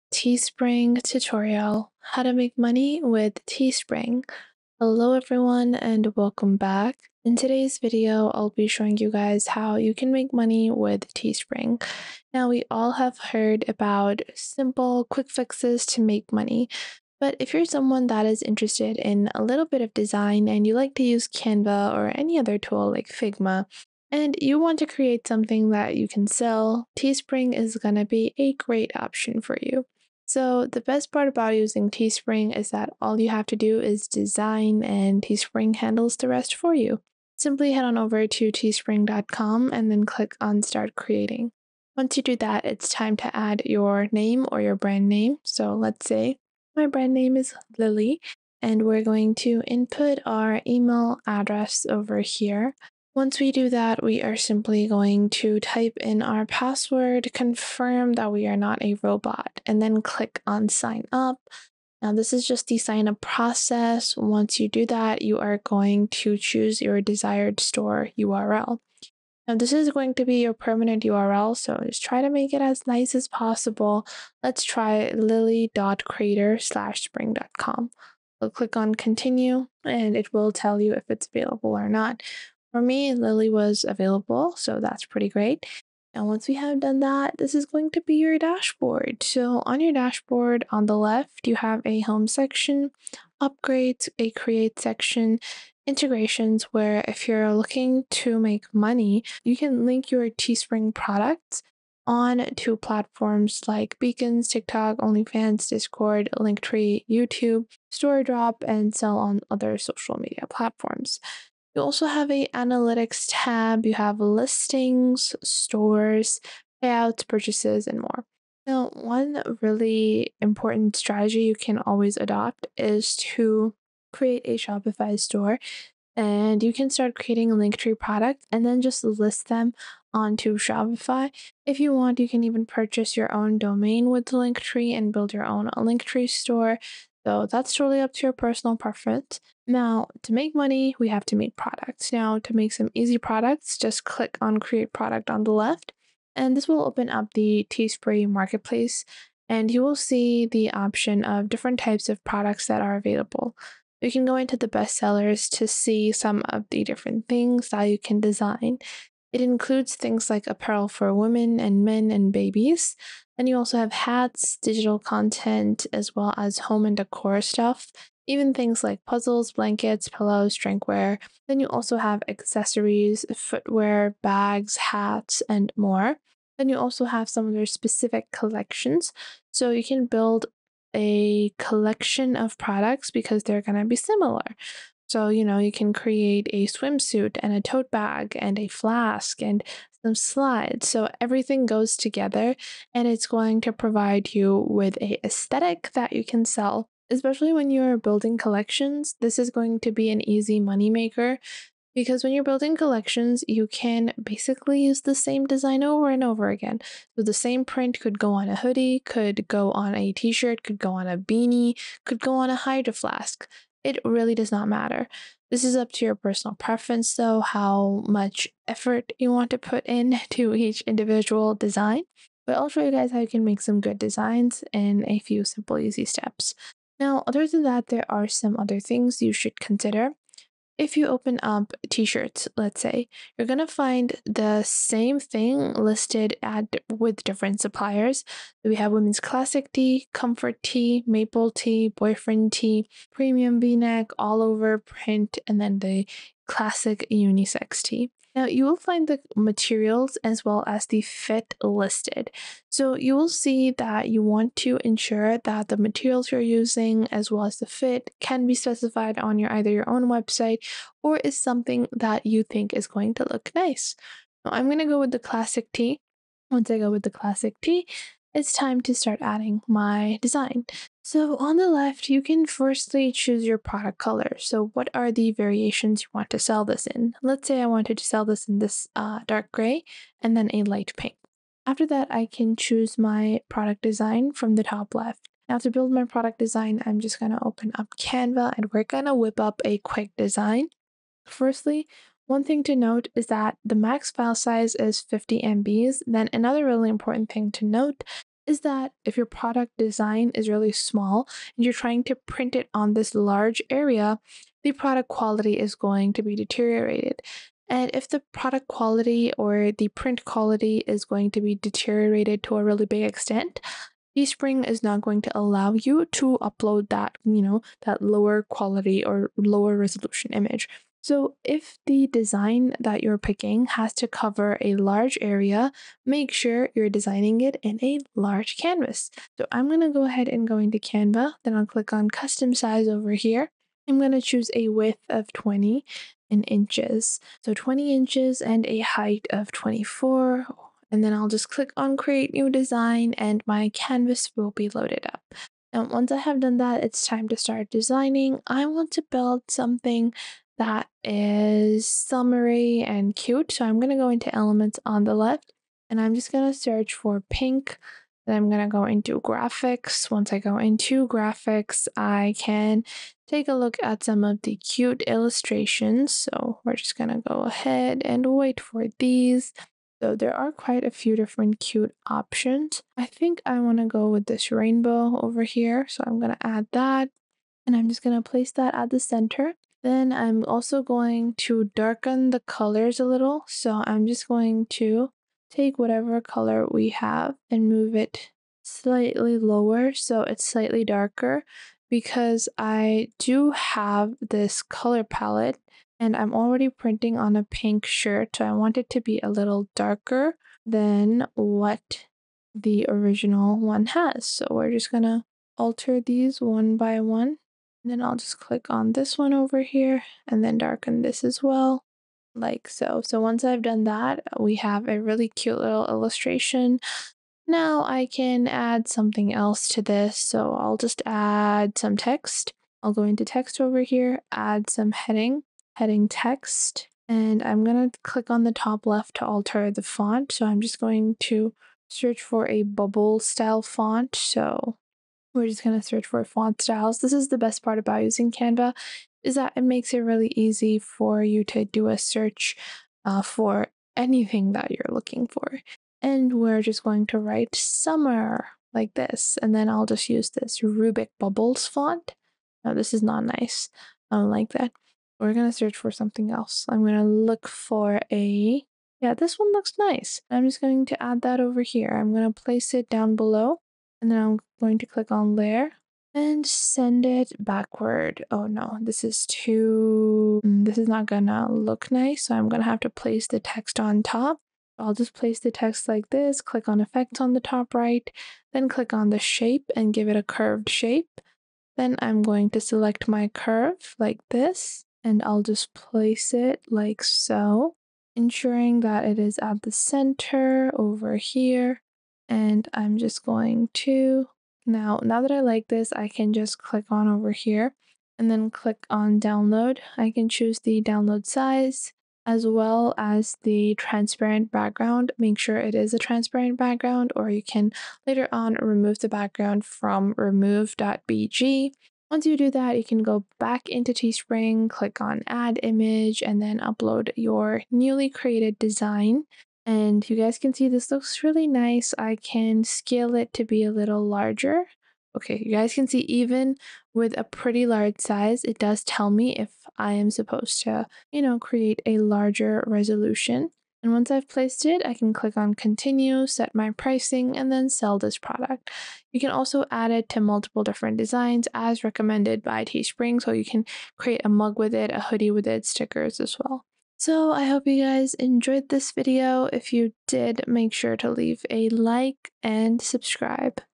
Teespring Tutorial: How To Make Money With Teespring (For Beginners)
In this video we show you How To Make Money With Teespring. This video will cover everything that you need to know and help you get started with this software.